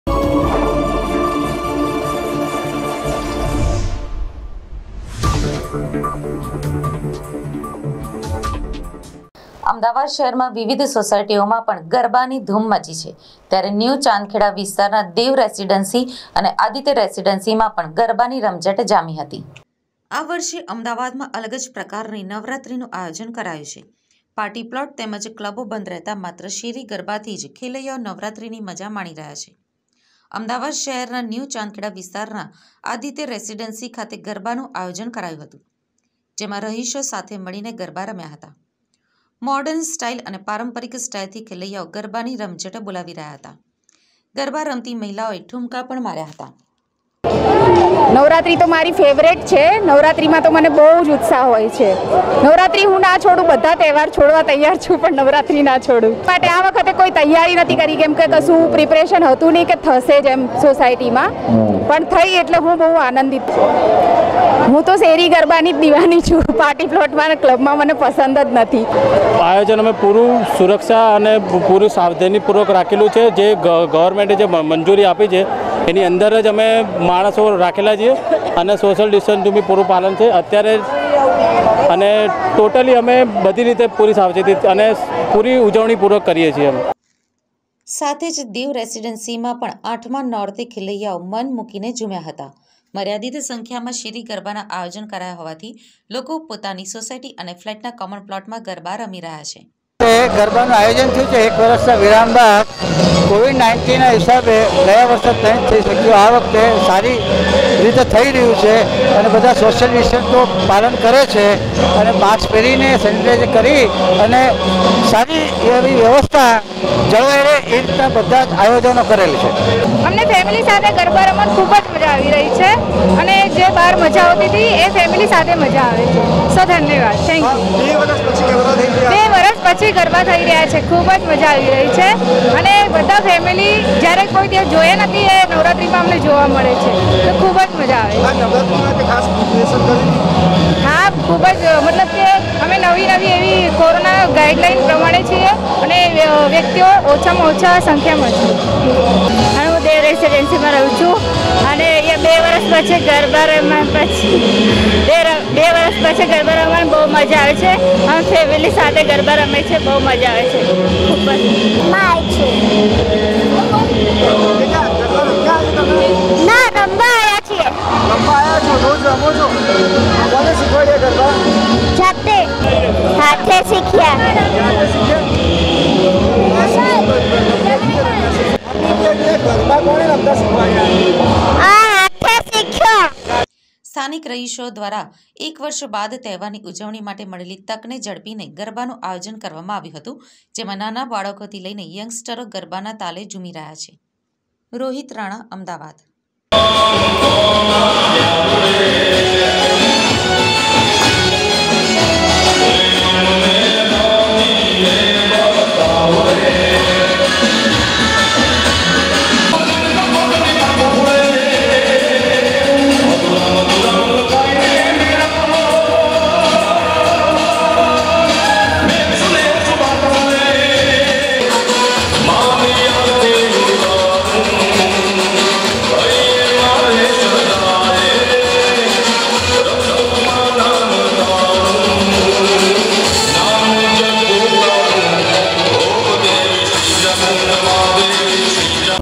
ન્યુ ચાંદખેડાના દેવ રેસીડેન્સી અને આદિત્ય રેસીડેન્સીમાં ગરબાની રમઝટ જામી
અમદાવાદ શહેરમાં વિવિધ સોસાયટીઓમાં પણ ગરબાની ધૂમ મચી છે.ત્યારે ન્યુ ચાંદખેડા વિસ્તારના દેવ રેસીડેન્સી અને આદિત્ય રેસીડેન્સીમાં પણ ગરબાની રમઝટ જામી હતી .